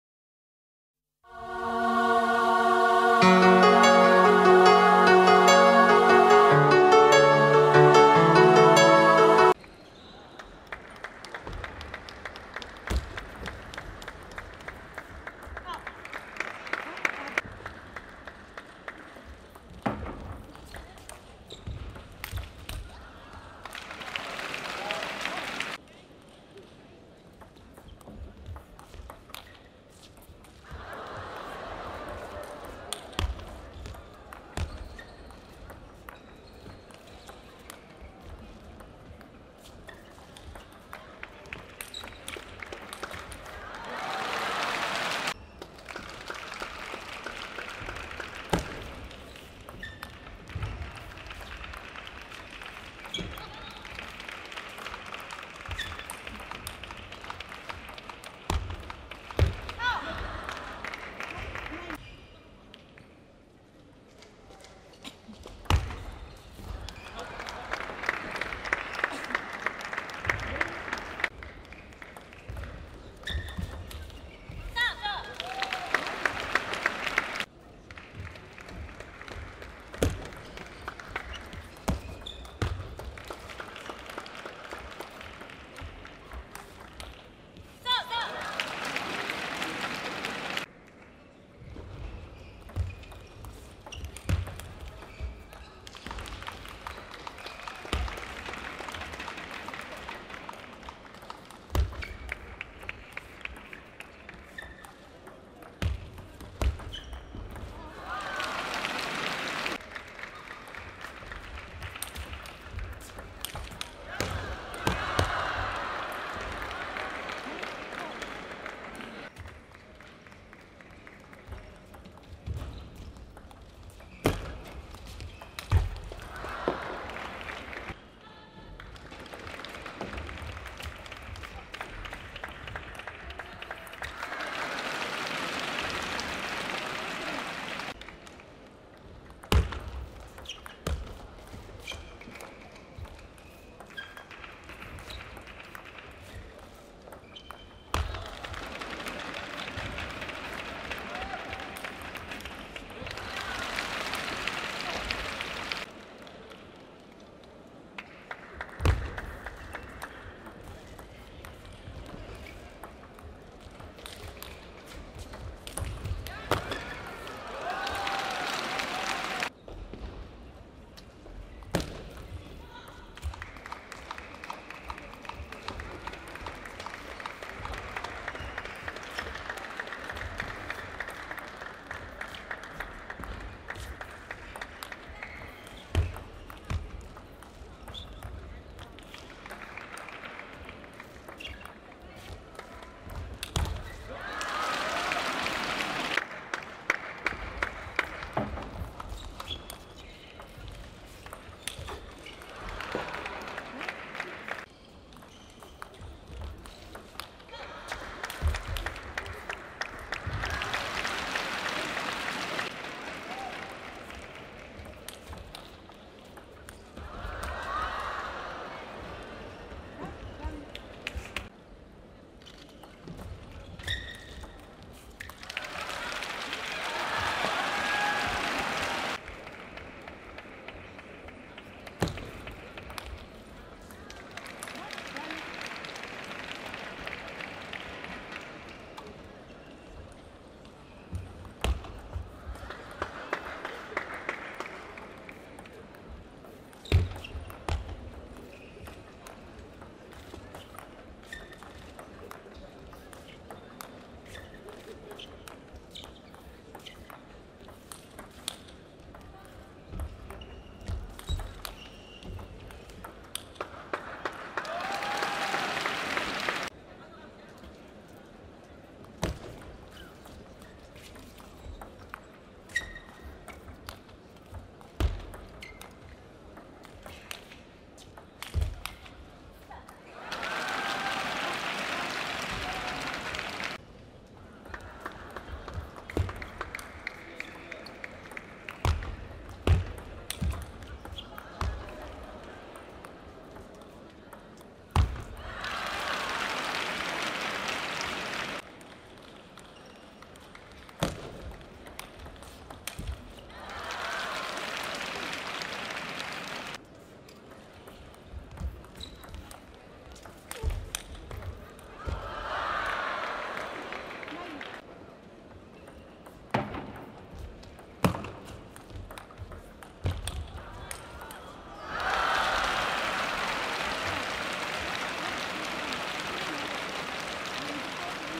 2011 Japan Open: Seo Hyo Won-Hirano Sayaka
2011 Japan Open Round of 32(8/7/2011):
Seo Hyo Won(KOR) vs Hirano Sayaka(JPN)